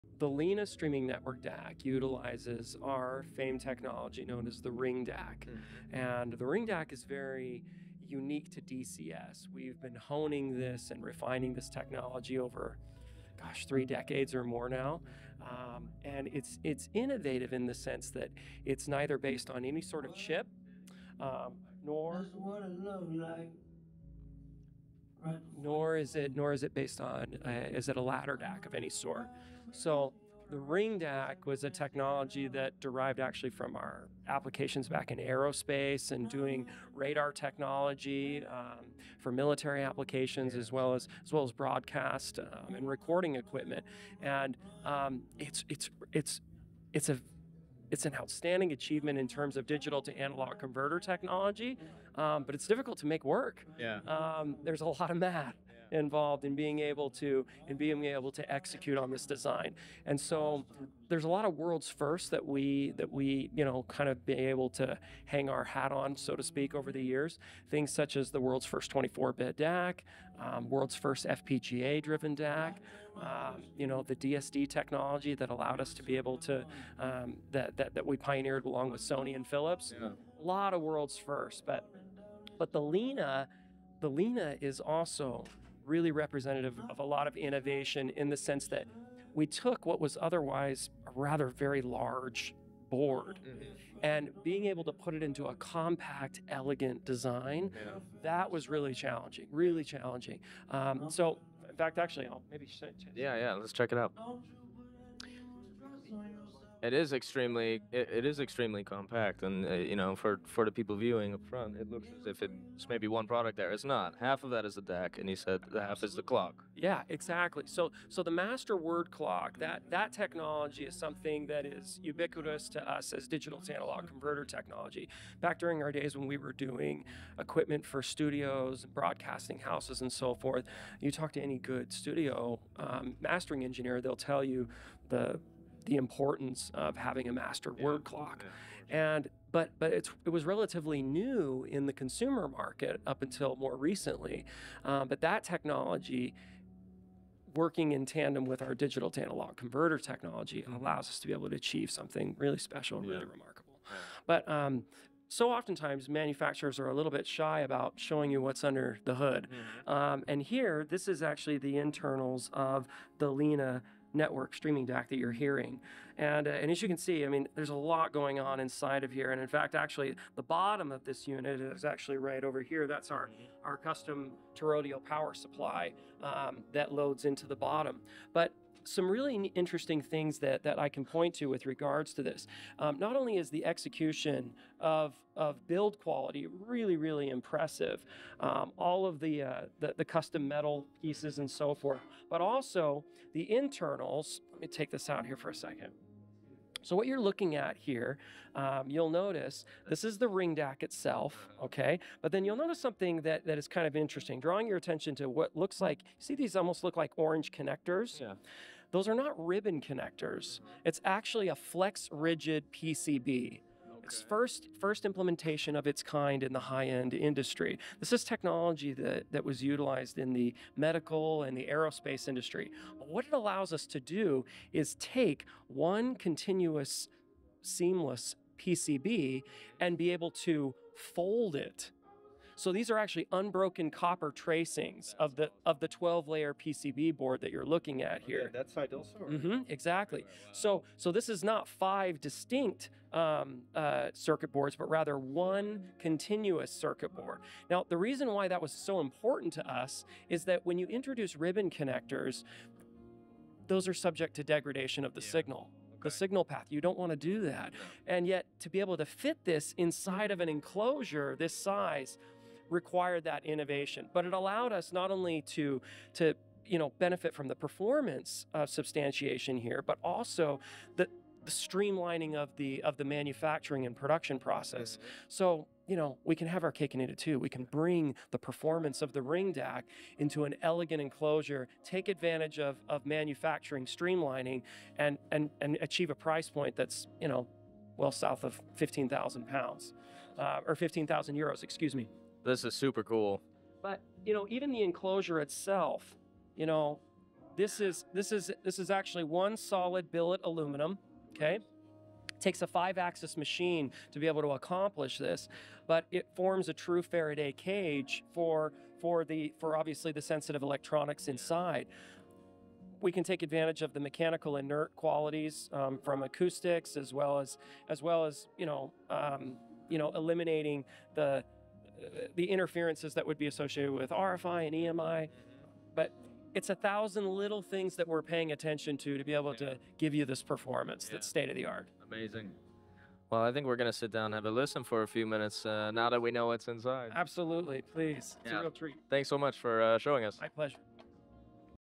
dCS Presents the LINA DAC System at High End Munich
The conversation discusses the features and technology behind the LINA streaming DAC, particularly emphasising the innovation of the Ring DAC technology by dCS. Emron highlights the challenges and achievements in designing the DAC, including its compact size and internal components like the custom power supply and flex rigid PCB (Printed Circuit Board). He explains how these components enhance performance and streamline manufacturing.

Special thanks to Emron Mangelson, JD (Head of Sales, North America) for taking the time to talk to us.